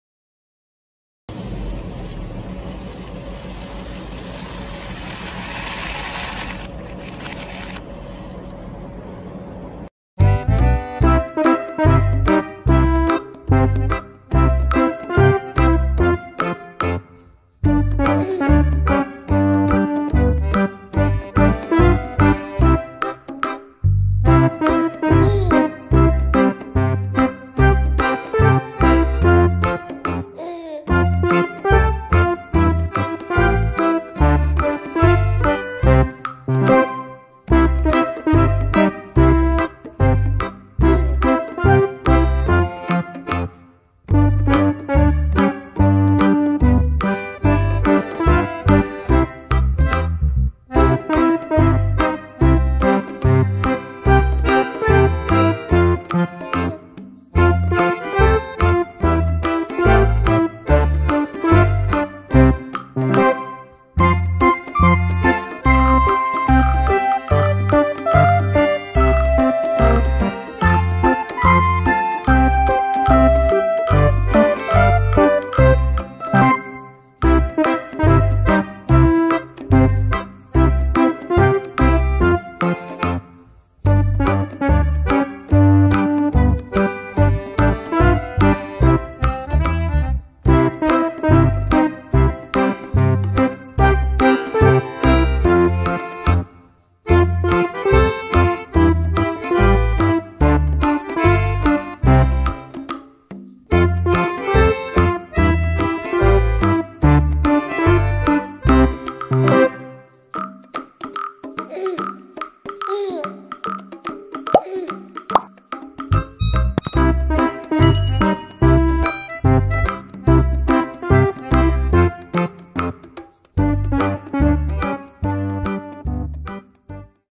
Peppa Pig Learns All About Romans | Puzzle for kids and for fun | Puzzle Lovers
Hello, Puzzle Lovers is with you and today we'll show you how to solve properly a puzzle called Peppa Pig Learns All About Romans.